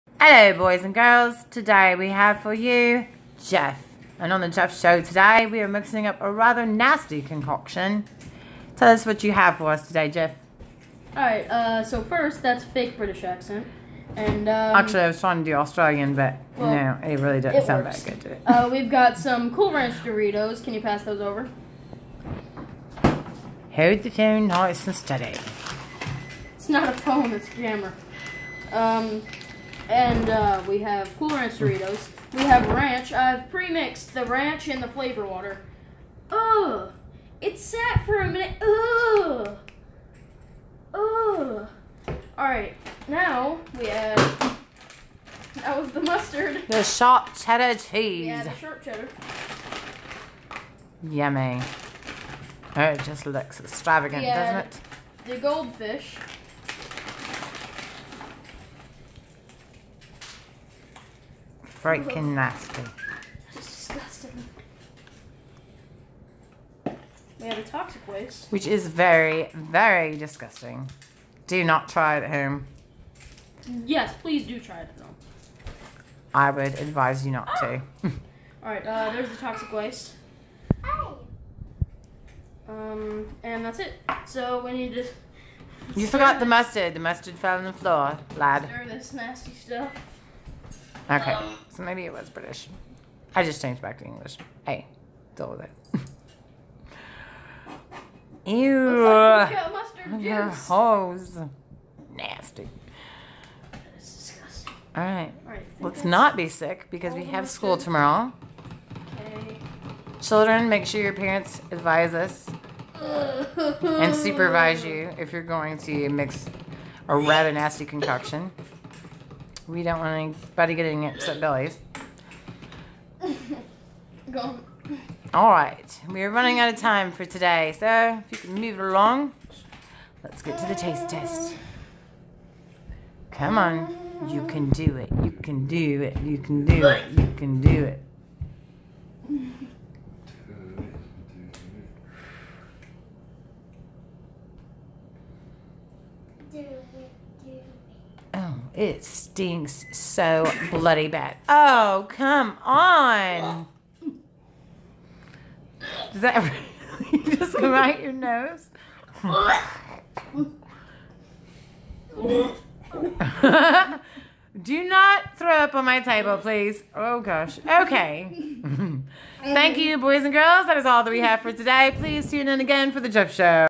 THE NASTIEST DRINK ON EARTH 'puked on table!'
I puked on my table while drinking the NASTIEST, stinkiest drink on earth.